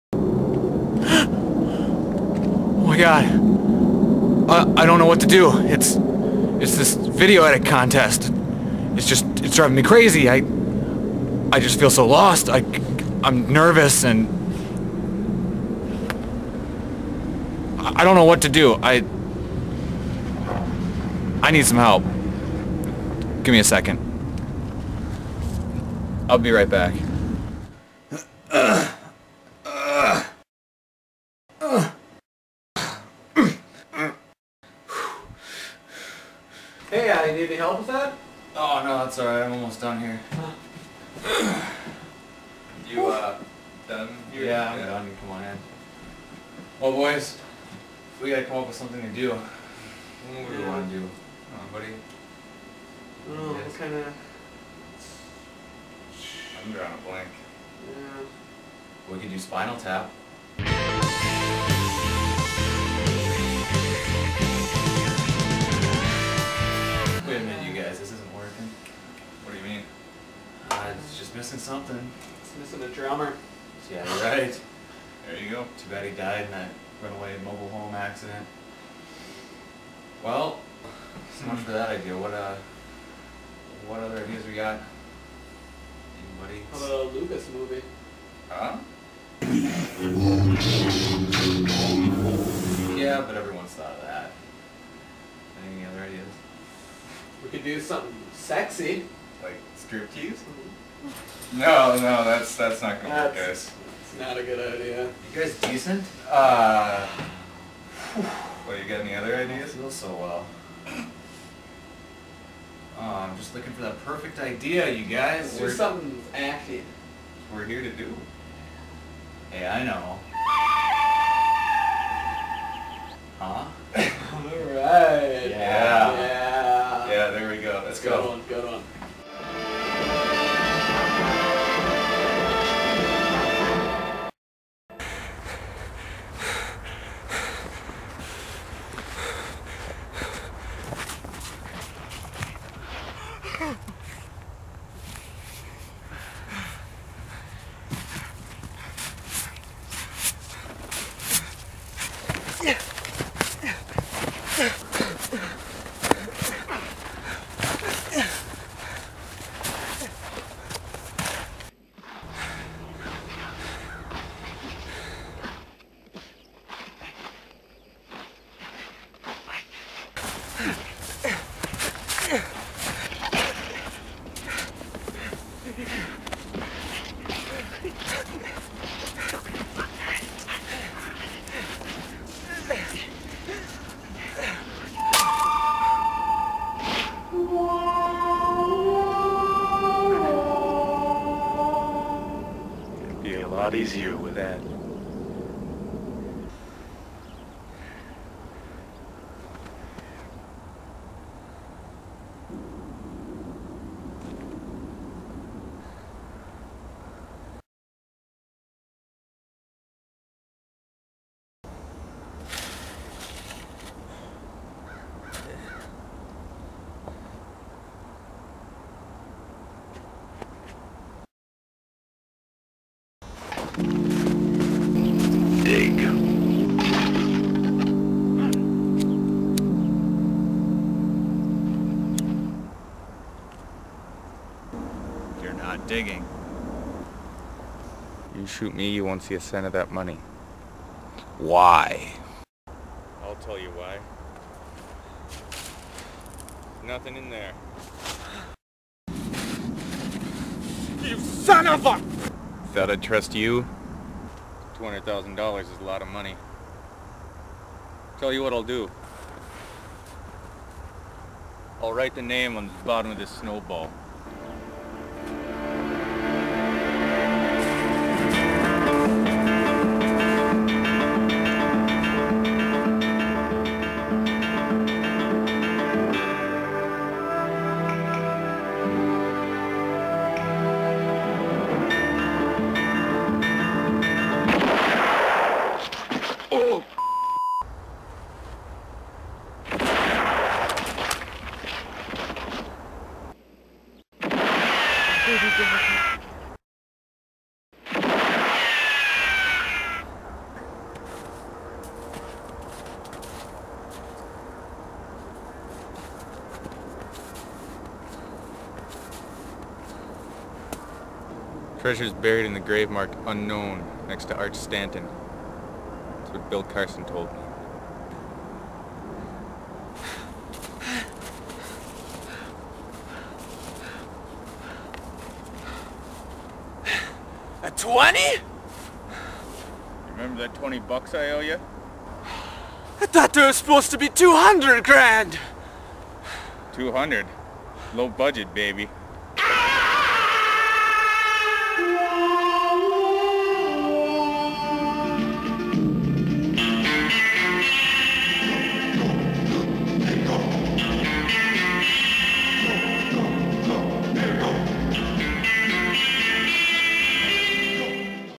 Fark Good, The Bad, & The Ugly
This vid was cobbled together in under 24 hours for a fark.com video edit contest. The contest theme was to make a low budget reproduction of a favorite movie.